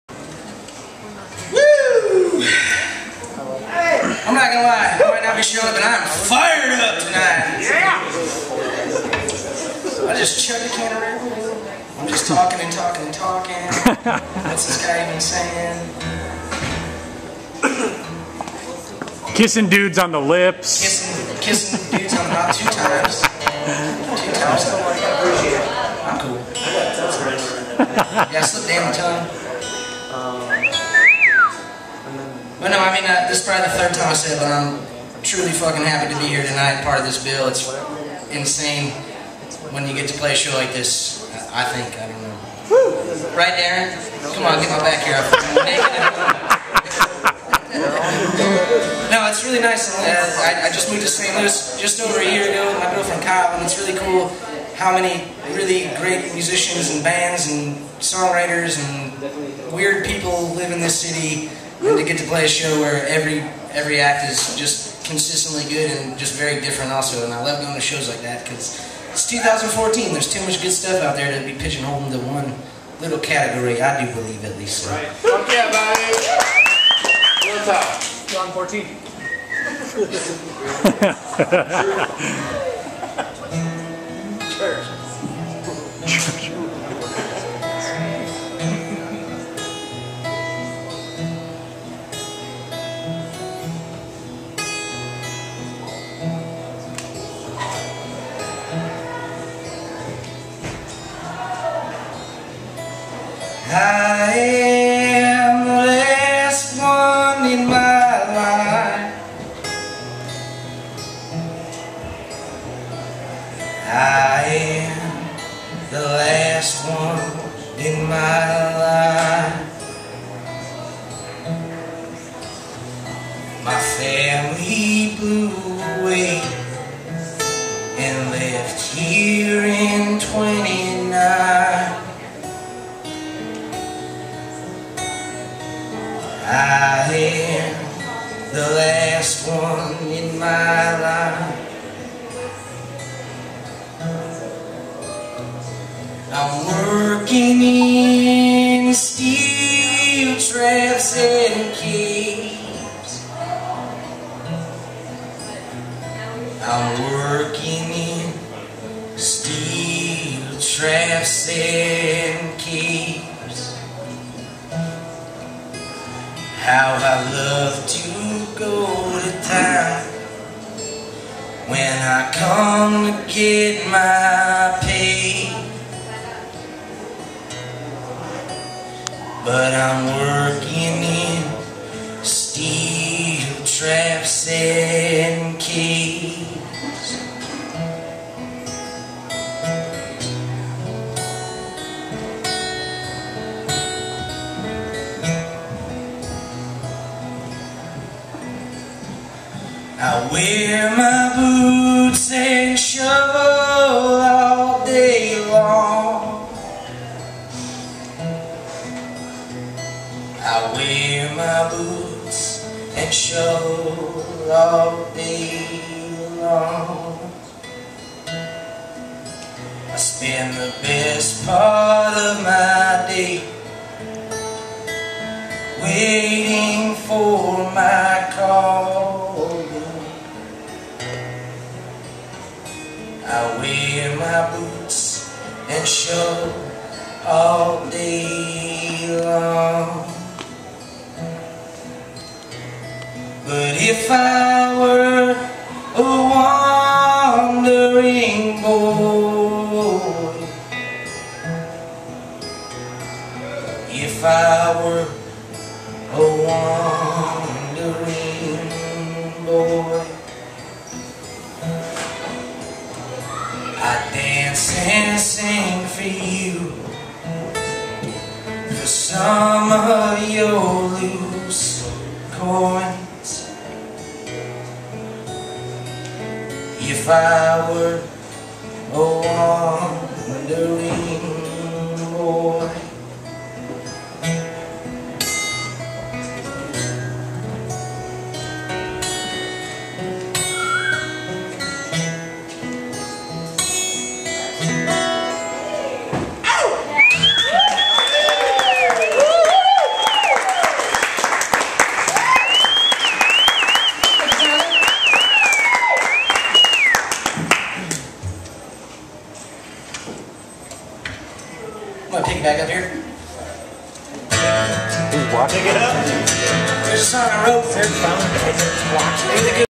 Bobby Stevens - Song 02 @ The Heavy Anchor 06/26/2014
Nice introduction speech, Bobby!

Oh yeah I thought Bobby was gonna play with a full backing band but nope!